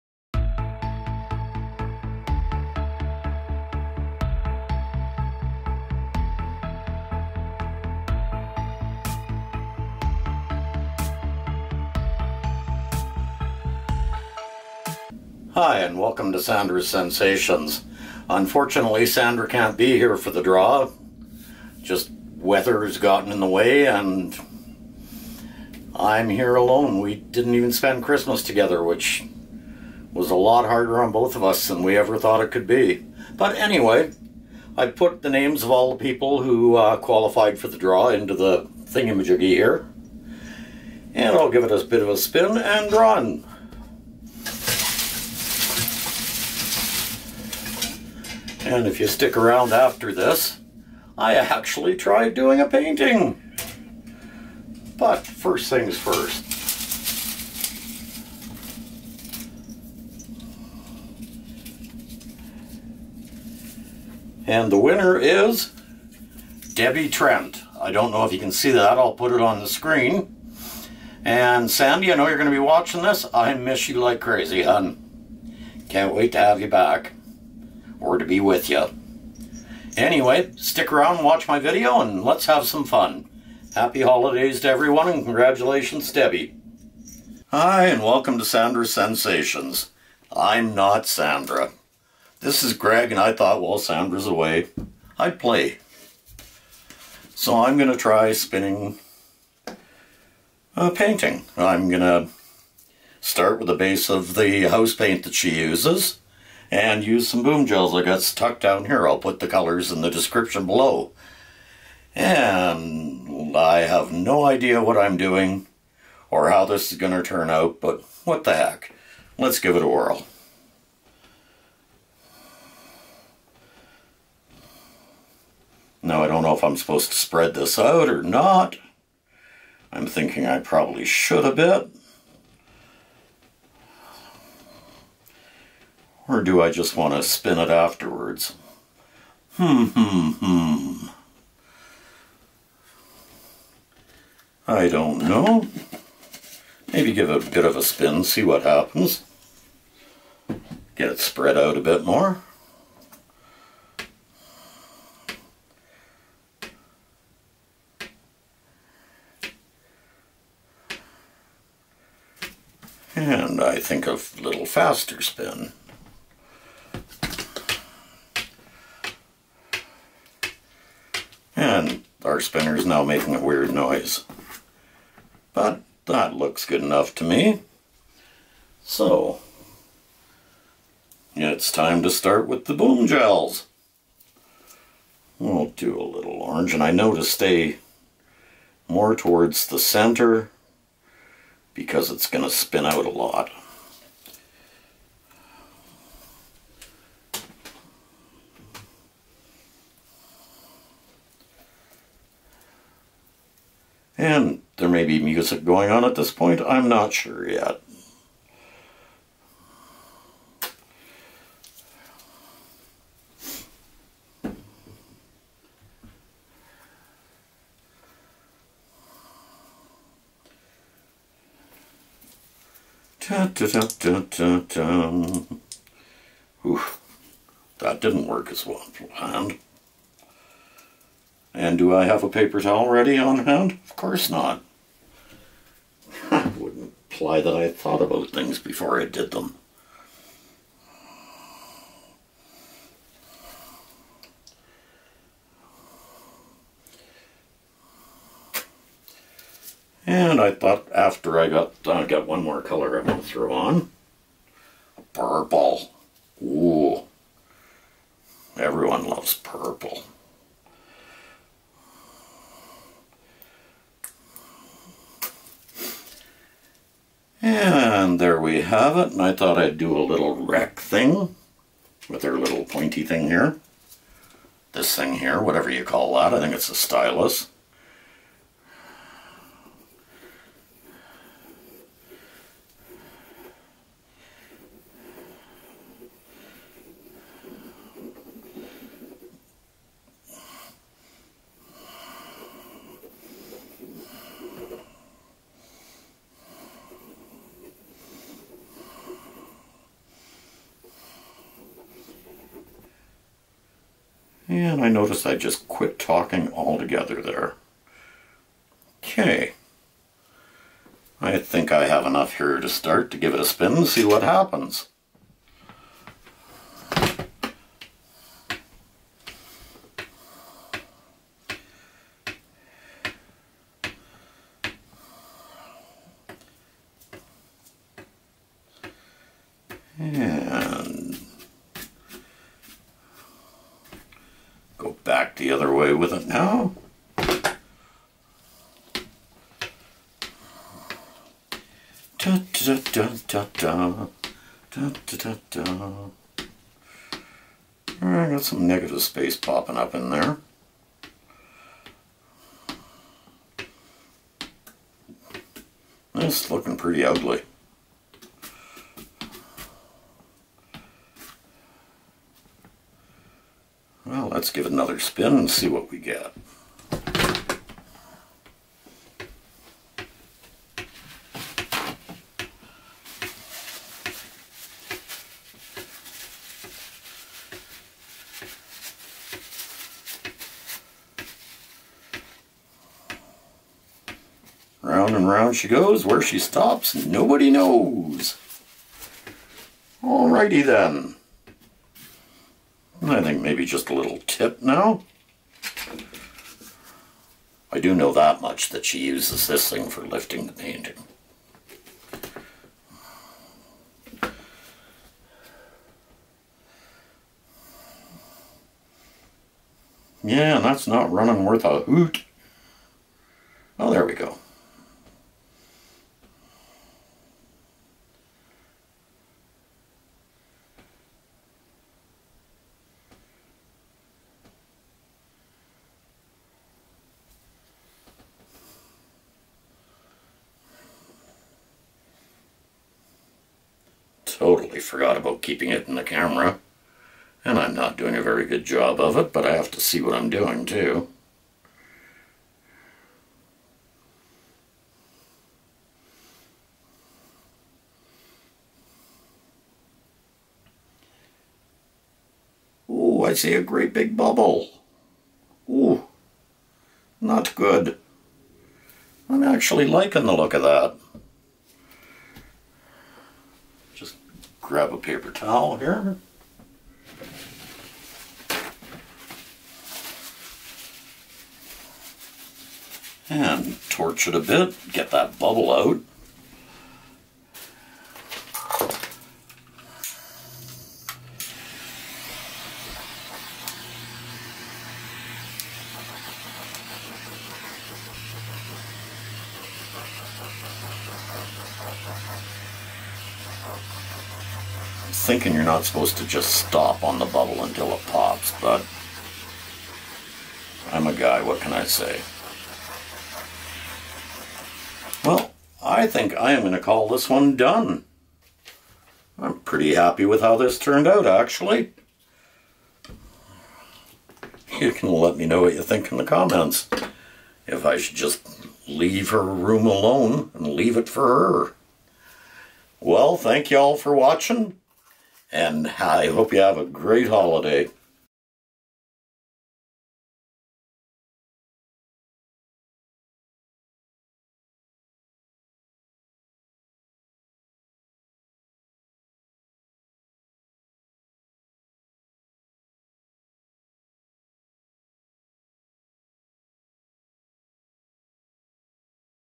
Contest Winner + Hubby Channel Highjack
Contest Winner + Hubby Channel Highjack - Tonight we announce the winner from Across The Globe 4/Kris Kringle Kiss collaboration. We had such fun doing that one! Oh yeah, after the announcement Greg highjacks my channel and shows his mad pour painting skills. He also wrote this description. :-)